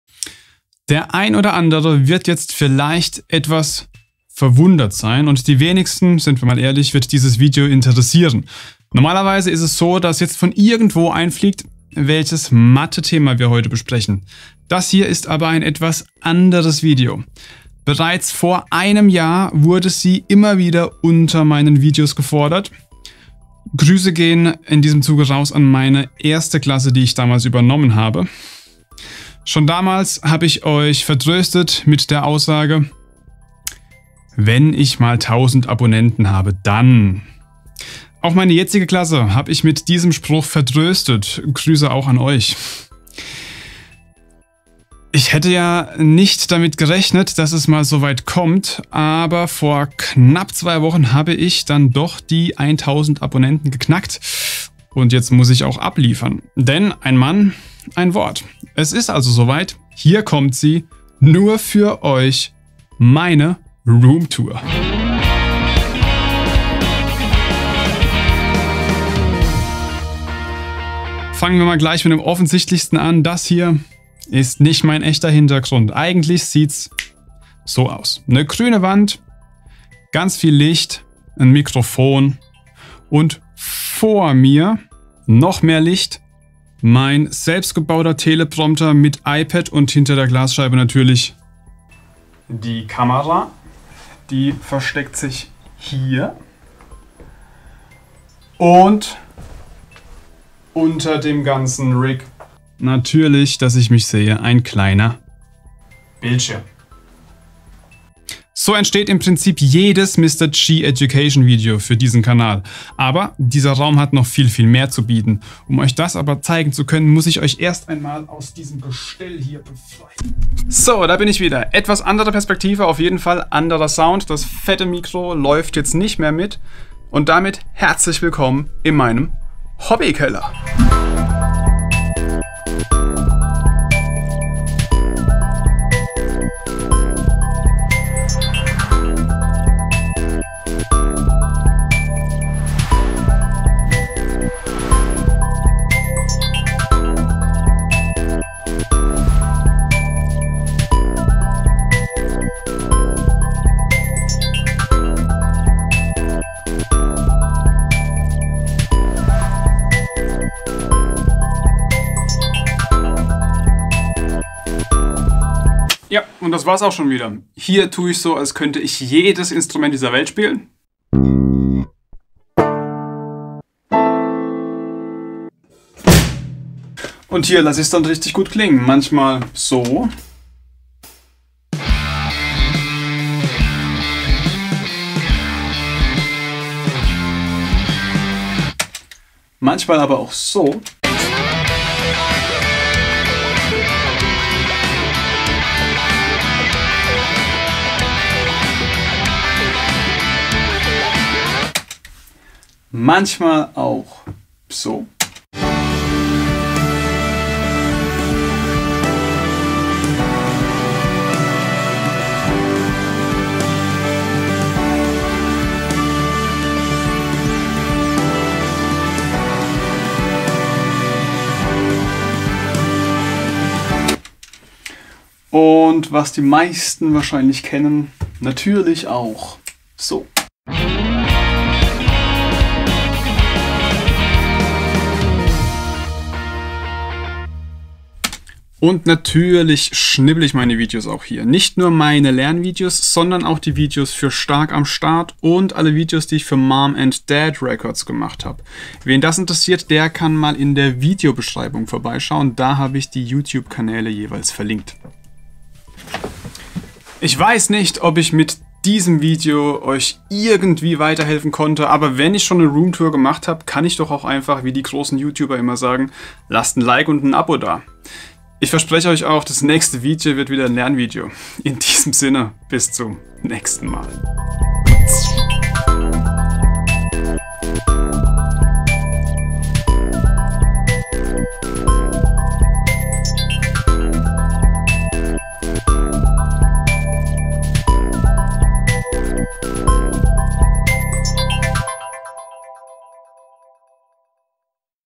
Mr. G - Die Roomtour 1k Special für meine Schüler
Versprochen ist versprochen und wird auch nicht gebrochen...
Jetzt ist es tatsächlich soweit. Mr G macht die Roomtour. So macht Mathe wieder Spaß